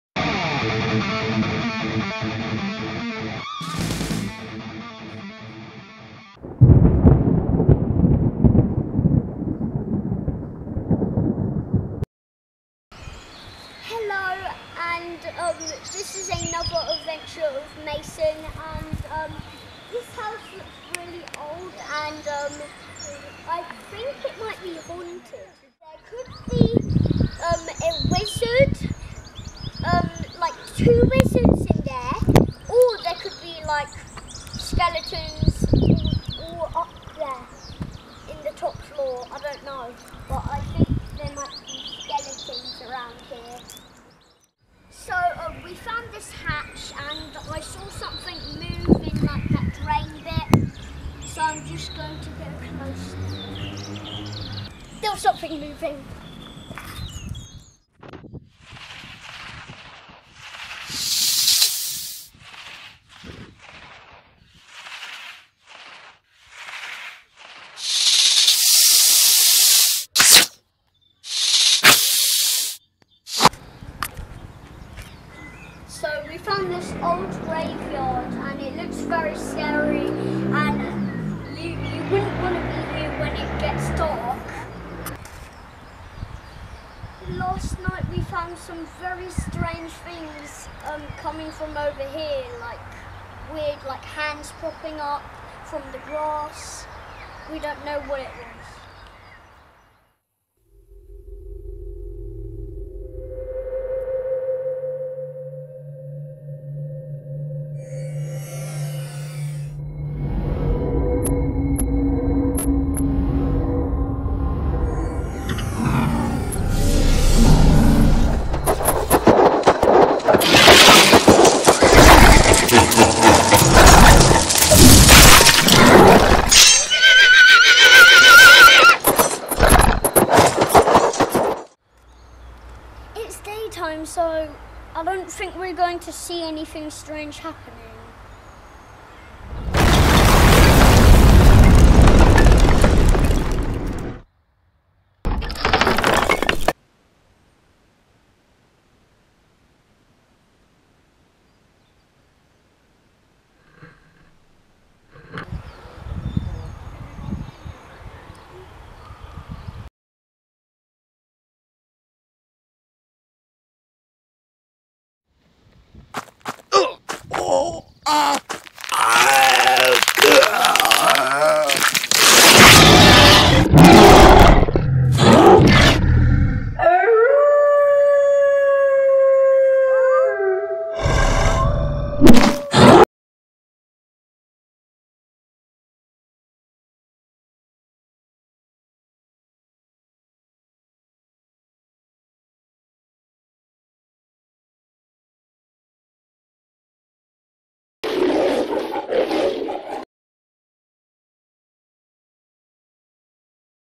Ghost Hunt - Adventures of Mason ep3
Mason visits a strange building where wizard's and Skeletons may be hiding during the day but something sinister is guarding the old building.
Mason then visits an old graveyard where strange things have been seen, will he see anything during the daytime?
There is an unexpected event on the way home that leads to Mason starting a new spooky investigation.